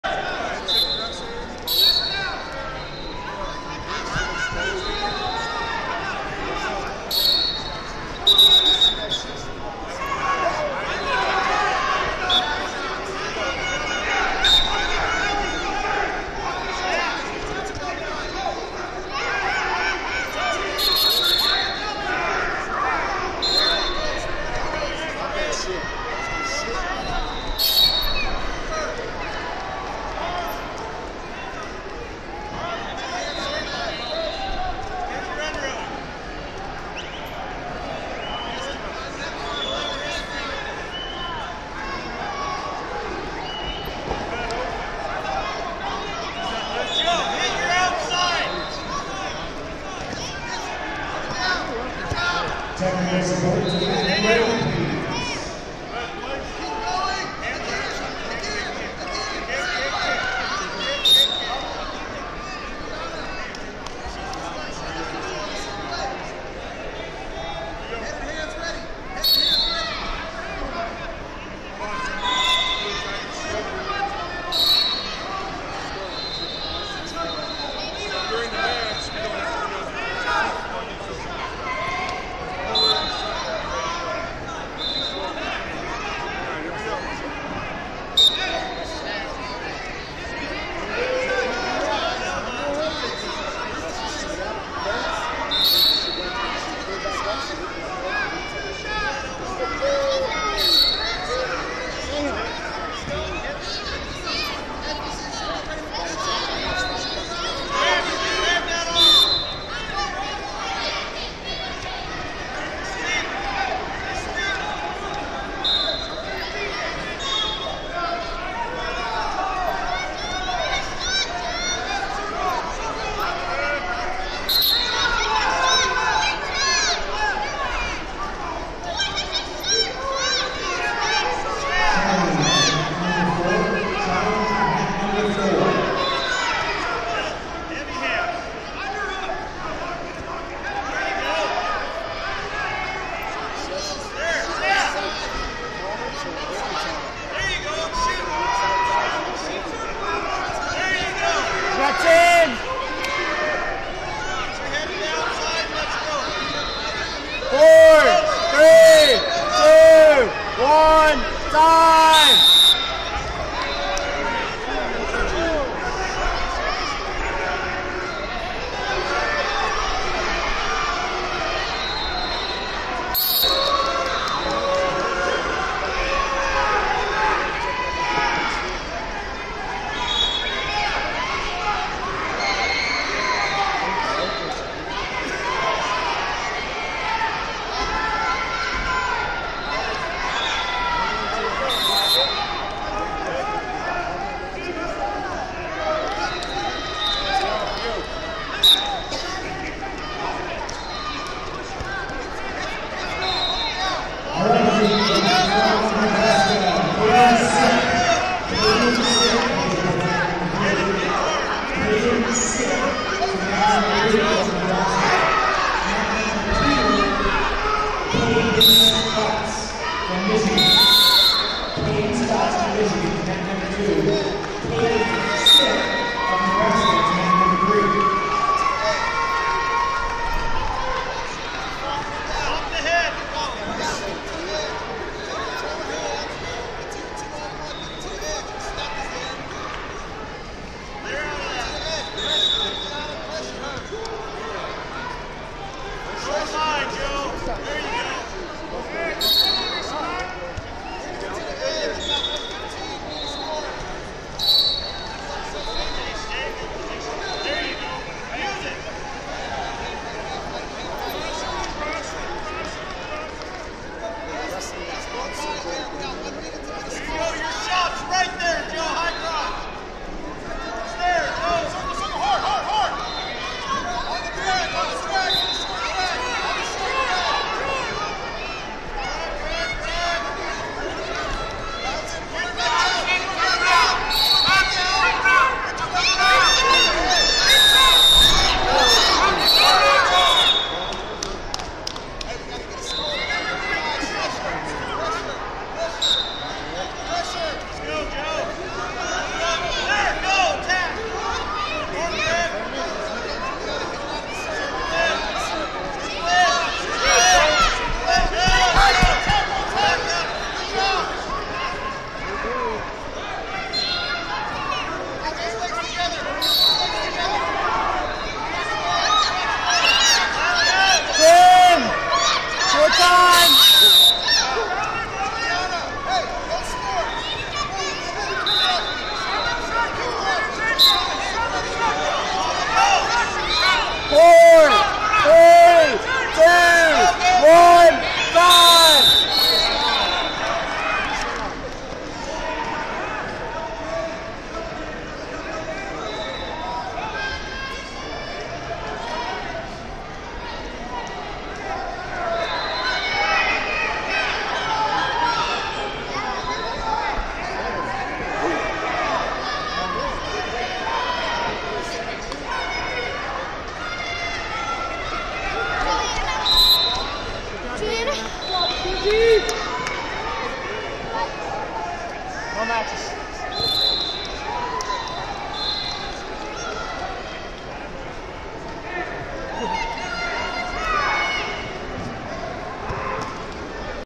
85 Lbs Cons. Round 2 - Sonja Hartle, WA Vs Julieanna Imke, WA 3704
Flosports-partner-access/2023 Women's National Championships & World Team Trials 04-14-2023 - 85 Lbs Cons. Round 2 - Sonja Hartle, WA Vs Julieanna Imke, WA 3704